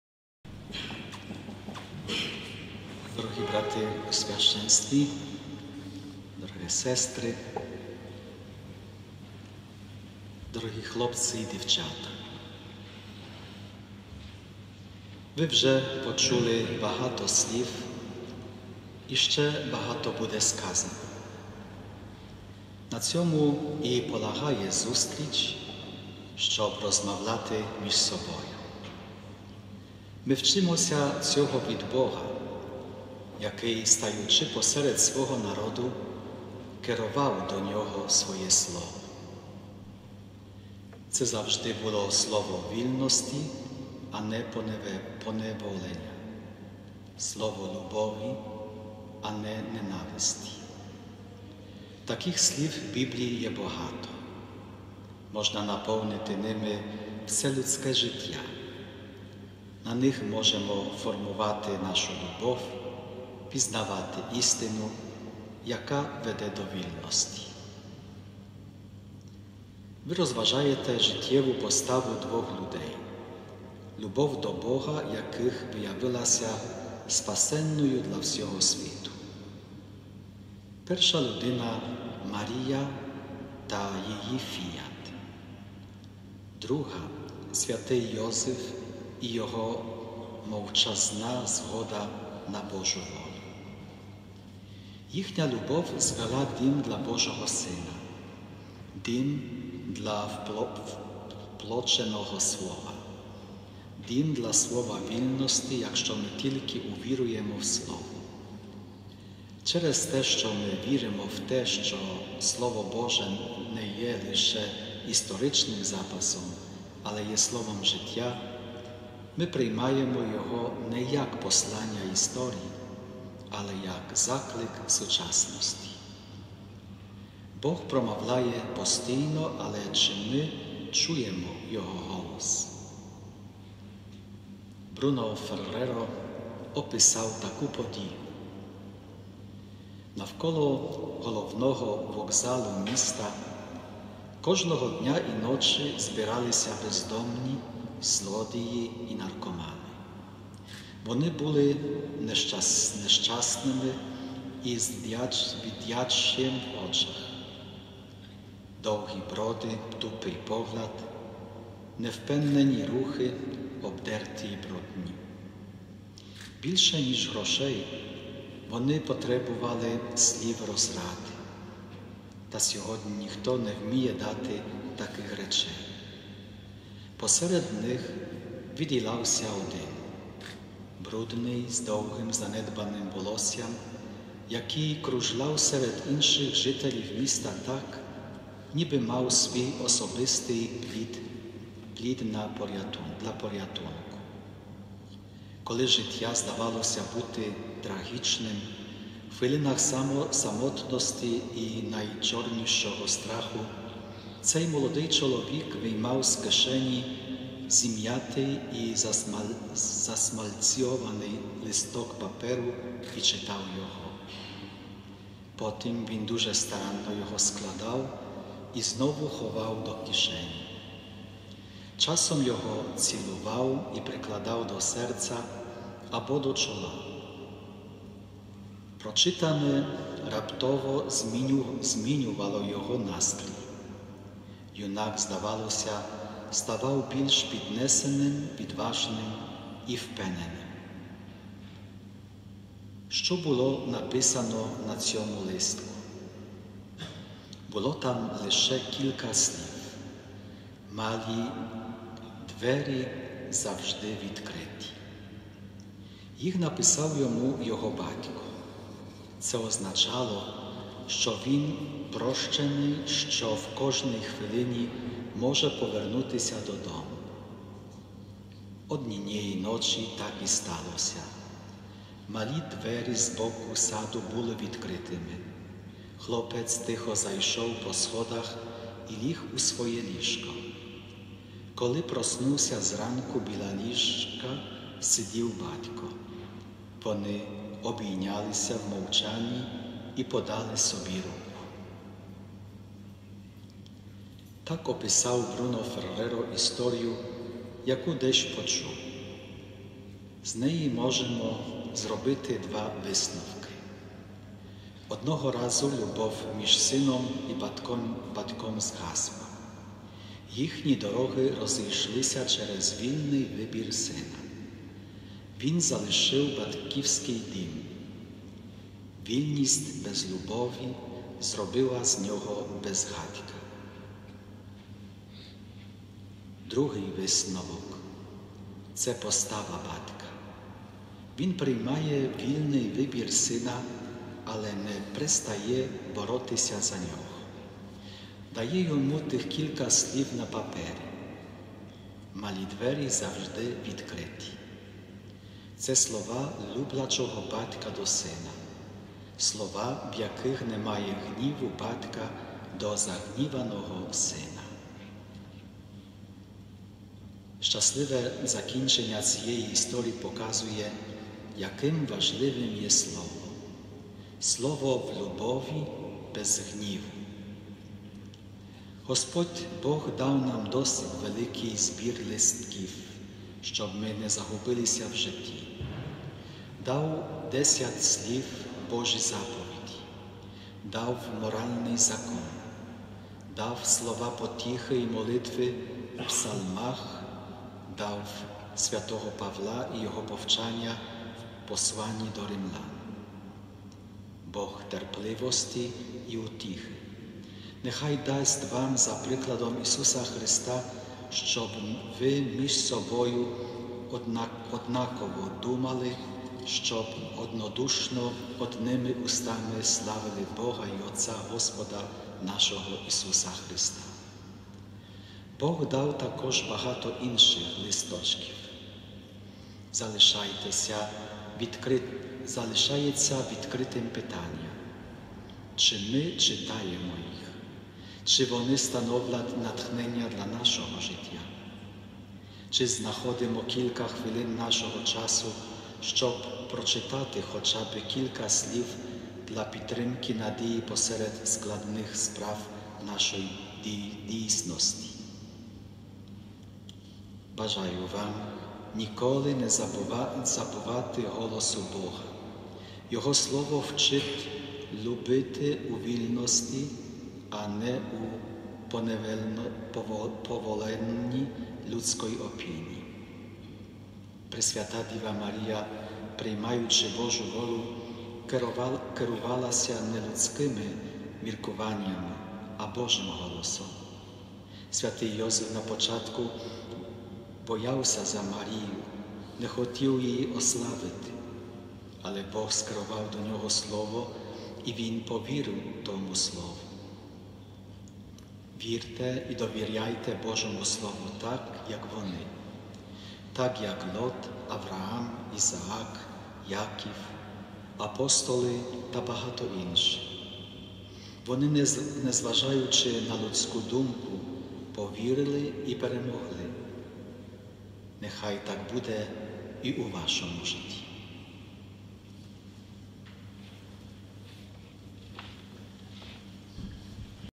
Львівській митрополит до молоді під час деканальної зустрічі
Деканальна зустріч молоді у Львівській катедрі.

27 березня 2021р. молодь із парафій Львівського деканату мали зустріч у Львівській катедрі, де спільно молились, розмовляли, слухали Слово Боже. Ця зустріч мала центральну тему «Любити — це вміти чекати».

Про любов Бога до людини, таку, що приносить плоди вічного життя, із молоддю говорив капуцин о. Сергій Воронюк, під час своєї конференції.

Час молитви та спілкування завершився спільною Святою Месою під проводом архієпископа Львівської архідієцезії Мечислава Мокшицького. У своєму слові до молоді єпископ говорив про Бога Отця, який має повноту життя і любові, яка перемагає будь-яке зло в житті людини, Його слово та діяння є сильніше від людської слабкості.